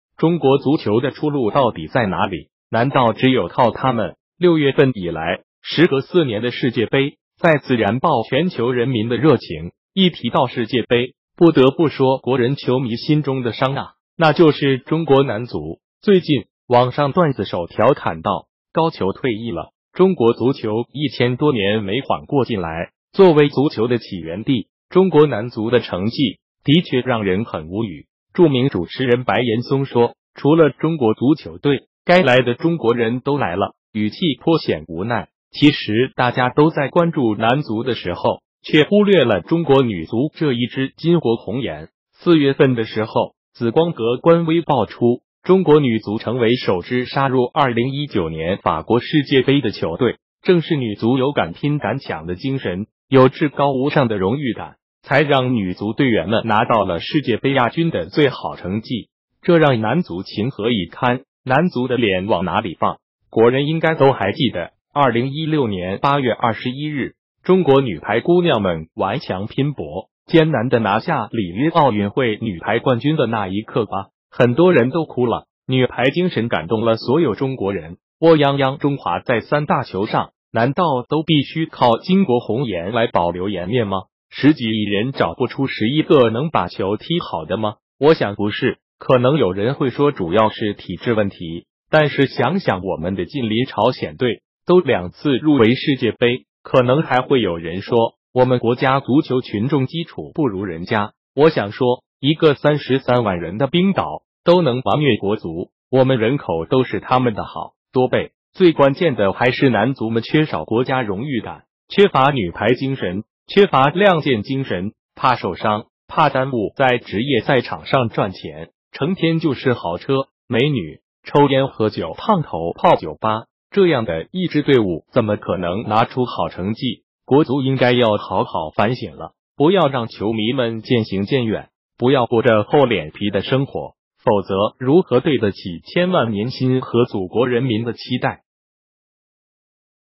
中國足球的出路到底在哪里？難道只有靠她們？
6月份以來，時隔四年的世界杯再次燃爆全球人民的熱情。一提到世界杯，不得不說國人球迷心中的傷啊，那就是中國男足。